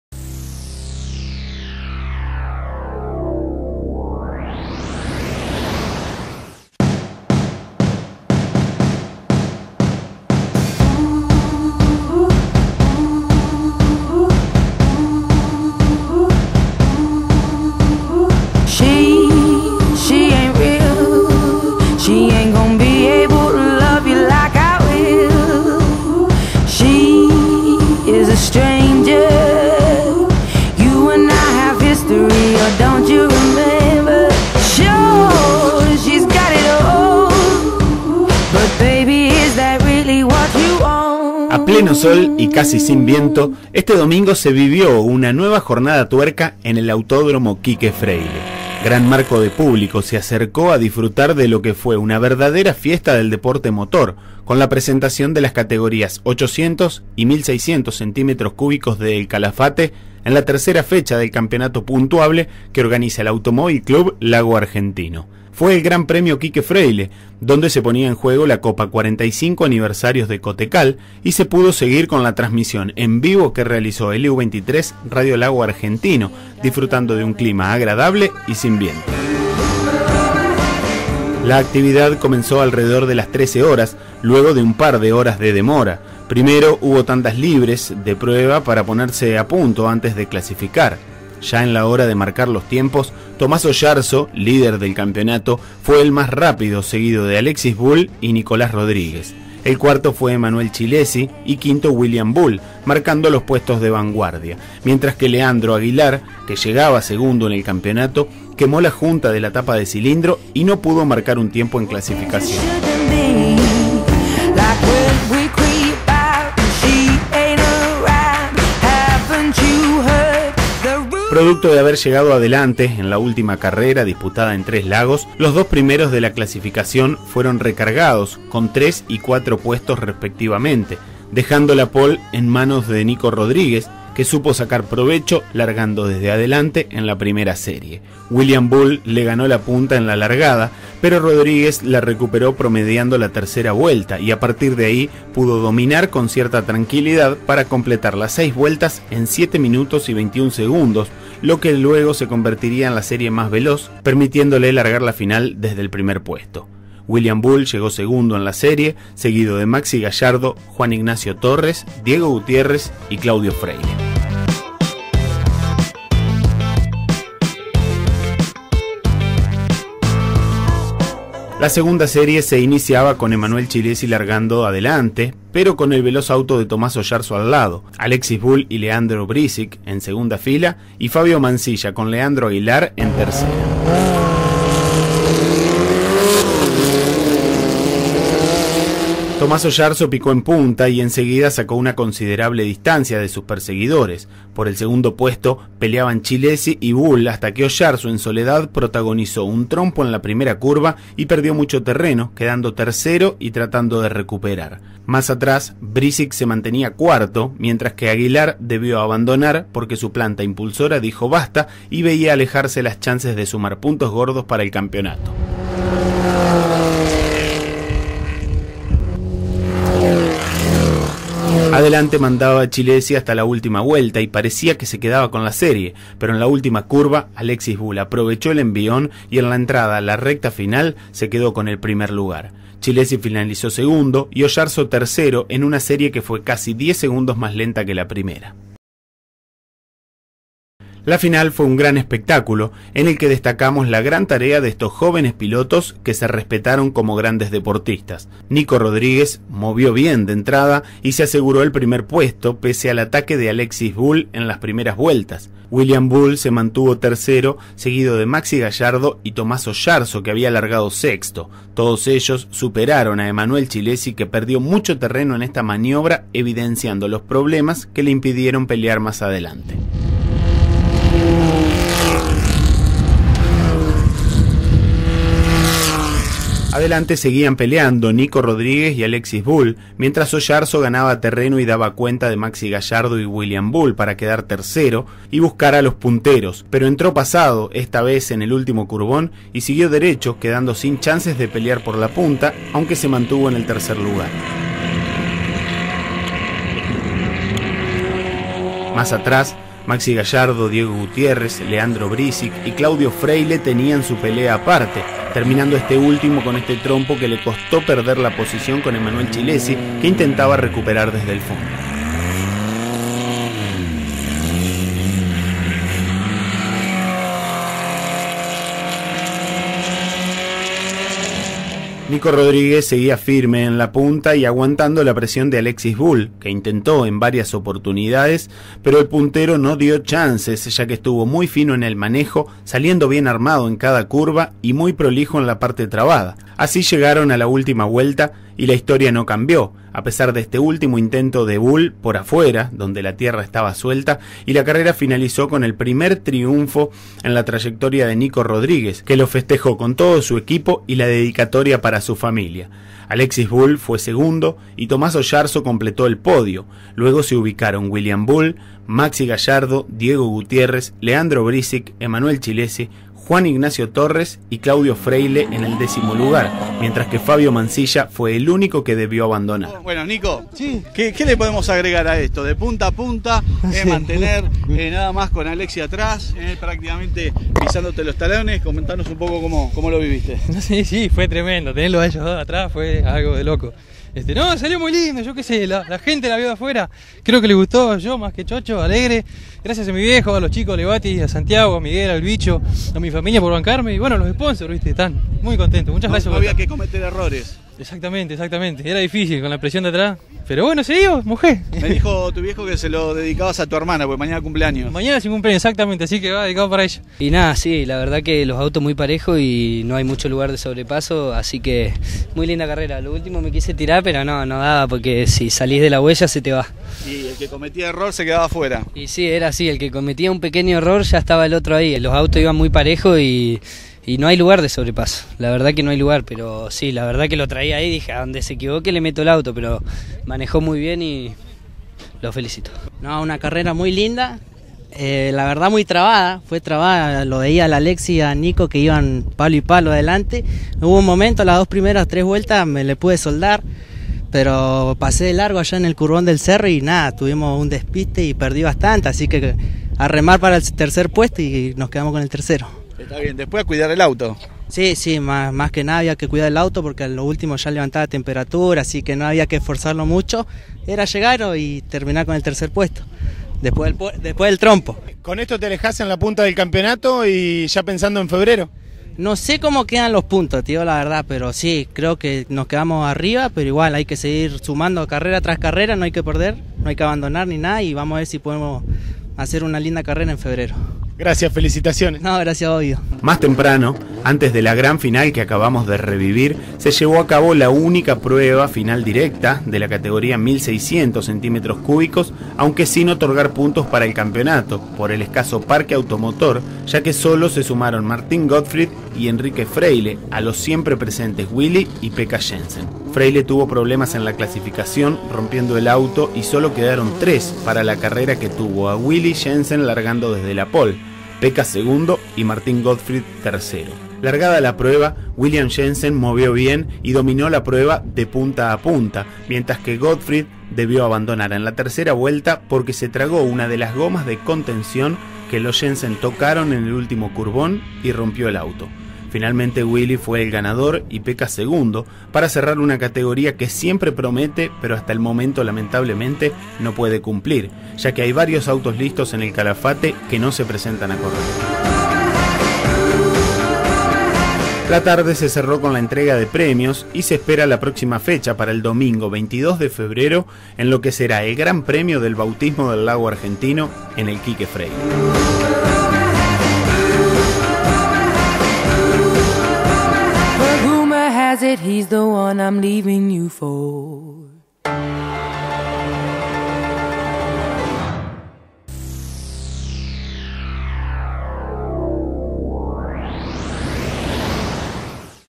Automovilismo - Copa 45° Aniversario CoTeCal
Nicolas Rodriguez se quedó con el primer puesto en la categoría 800cc. de punta a punta, en un gran espectáculo que incluyó a Alexis Bull y Tomas Oyarzo en el podio.